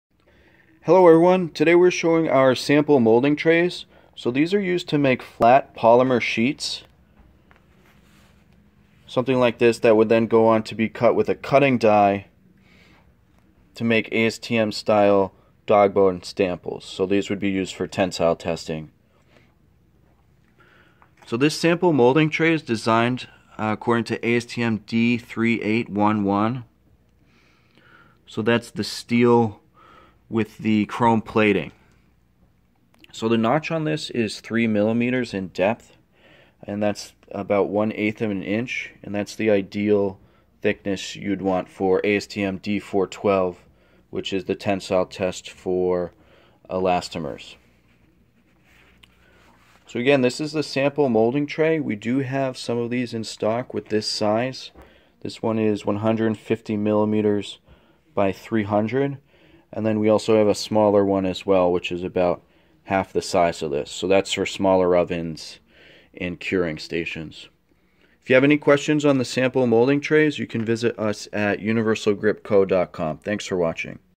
Sample Molding Trays for Casting ASTM Specimen Sheets
The Sample Molding Trays are used to create thin sheets of Polymer in a lab. The freshly mixed polymer is poured onto the tray. The tray is then usually put inside an oven so it can cure for a required length of time. Once the sheet is done curing, it is removed from the tray and cut with the appropriate cutting die.

These trays are made with chrome plated steel. Depending on the exact polymer, a release agent may need to be used. We stock trays in either 300x150mm or 150x150mm sizes.